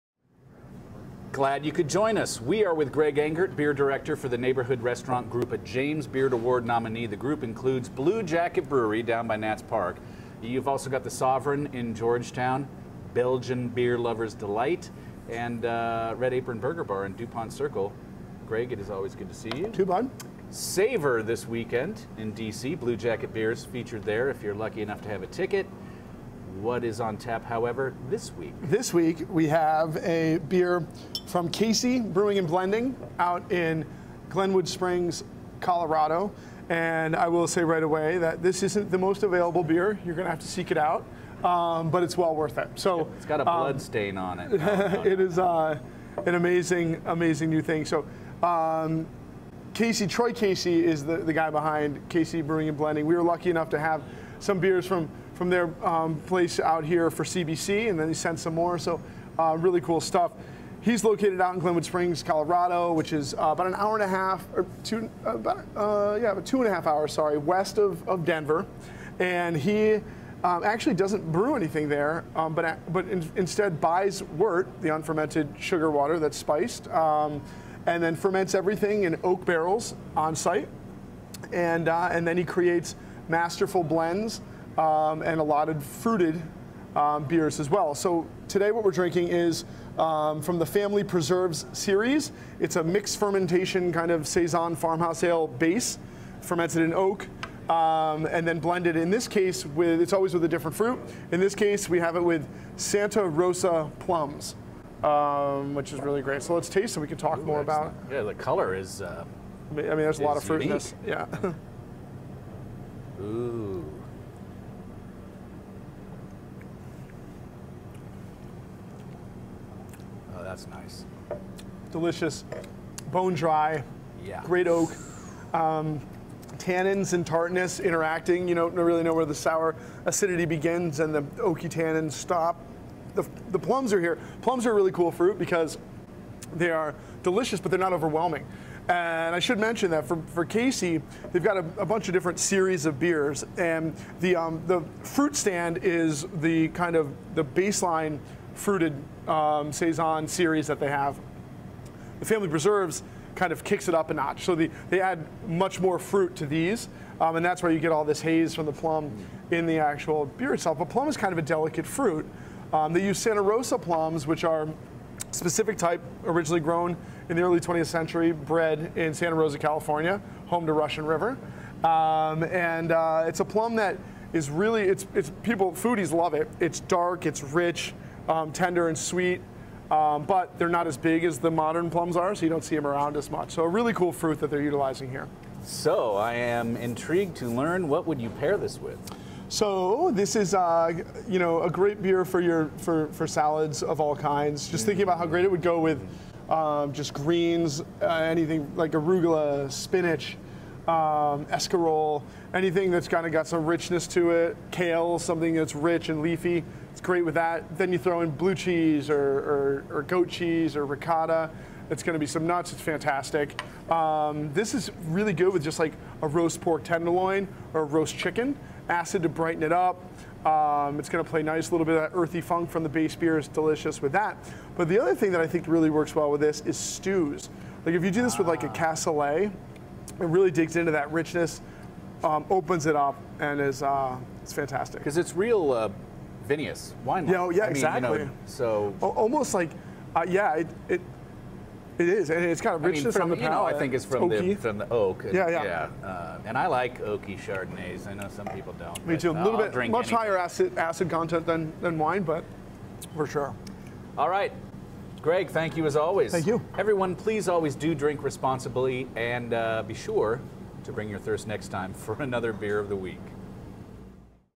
WTOP's Beer of the Week (June 3rd): Casey Family Preserves Plum
WASHINGTON -- Seeing plums in a whole new liquid light!

WTOP's Brennan Haselton is joined by Neighborhood Restaurant Group Beer Director and Food and Wine Sommelier of the Year Greg Engert to talk about Casey Family Preserves Plum for the latest edition of WTOP's Beer of the Week.